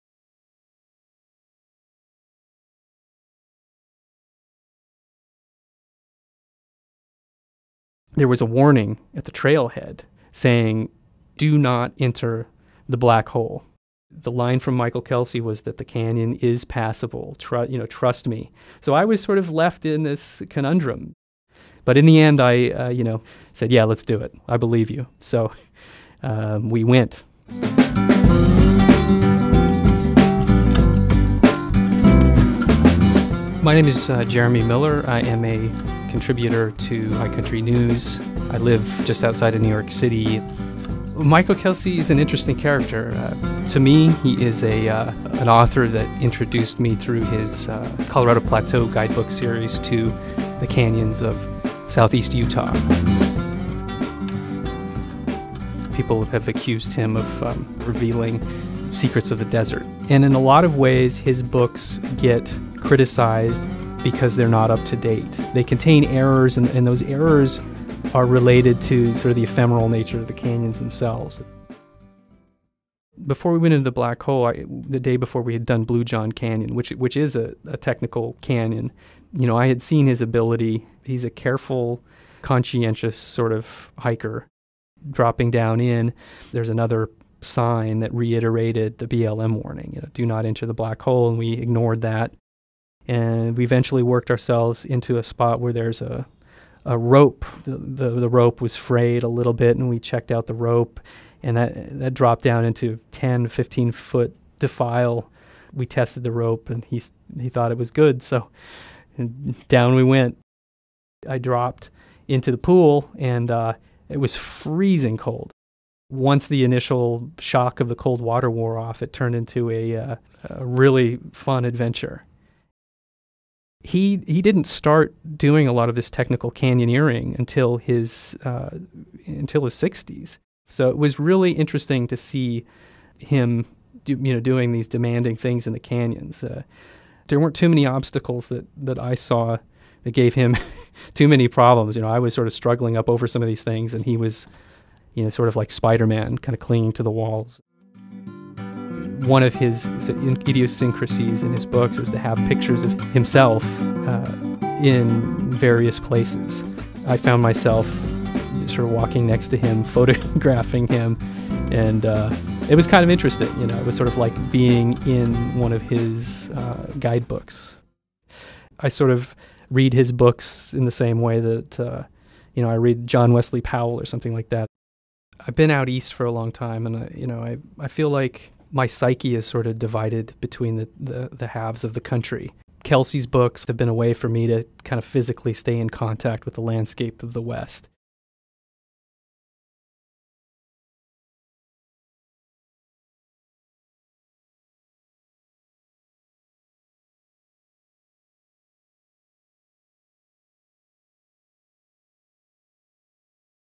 Down the Black Hole
This summer, journalist Jeremy Miller descended two notorious Utah slot canyons with an equally notorious guide.